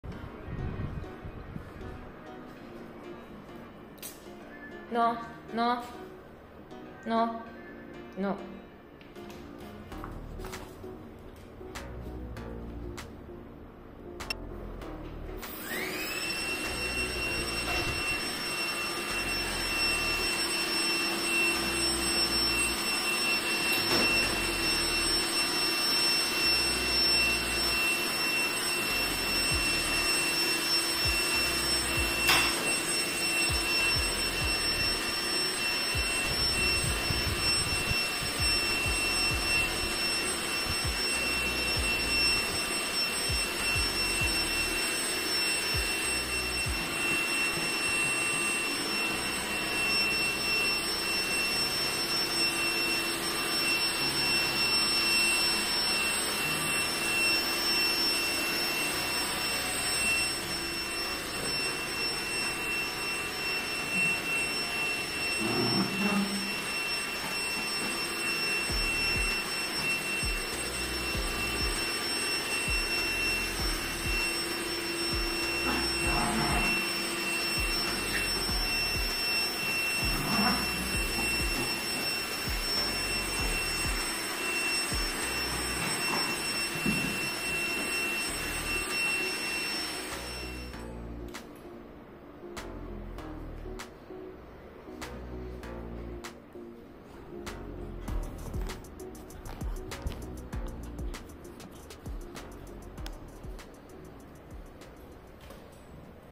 Beagle Puppy wants to turn on Vacuum & Play with it - Beagle Puppy Funny Video
Beegi Vlog 76 - Beagle Puppy wants to turn on Vacuum & Play with it - Beagle Puppy Funny Video